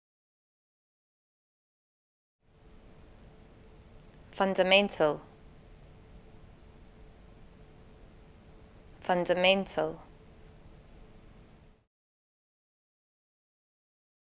How to pronounce FUNDAMENTAL in British English
This video shows you how to pronounce FUNDAMENTAL in British English. Speaker has an accent from Glasgow, Scotland. Collins Dictionary, the home of living English and pioneers of dictionary publishing